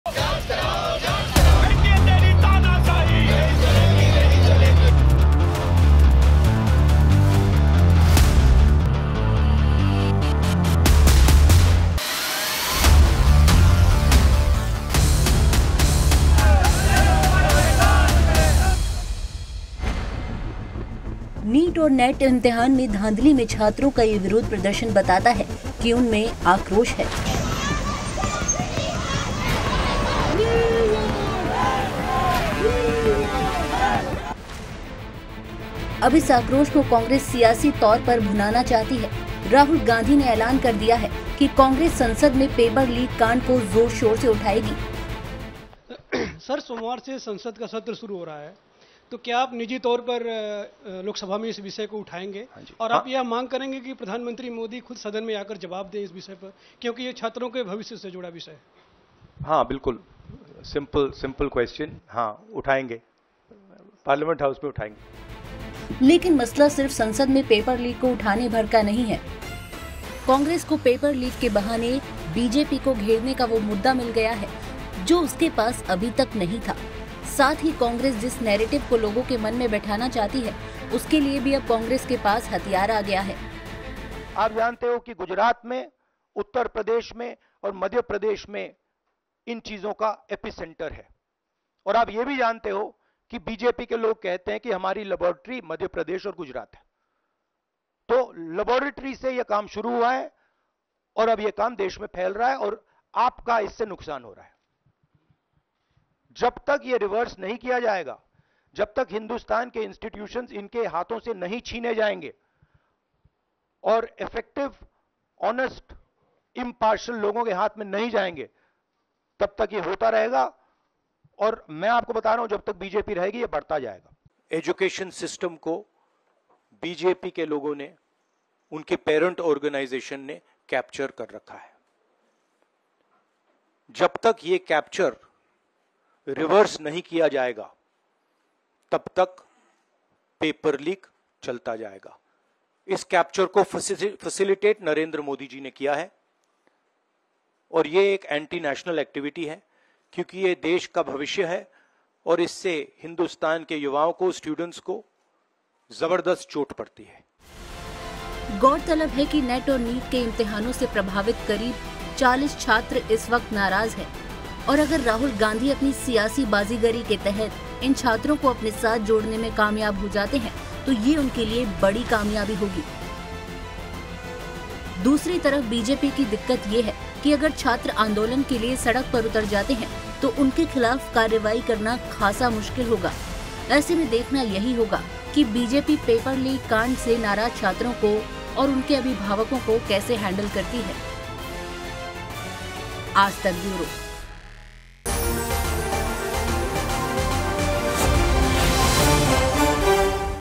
Paper Leak पर Rahul Gandhi का BJP पर ताबड़तोड़ आरोप, क्या मुश्किल में फँस गई सरकार? Chunav Aaj Tak
नीट और नेट इम्तिहान में धांधली के खिलाफ छात्रों में आक्रोश है। अब इस आक्रोश को कांग्रेस सियासी तौर पर भुनाना चाहती है। राहुल गांधी ने एलान कर दिया है कि कांग्रेस संसद में पेपर लीक कांड को जोर-शोर से उठाएगी। देखिए ये रिपोर्ट।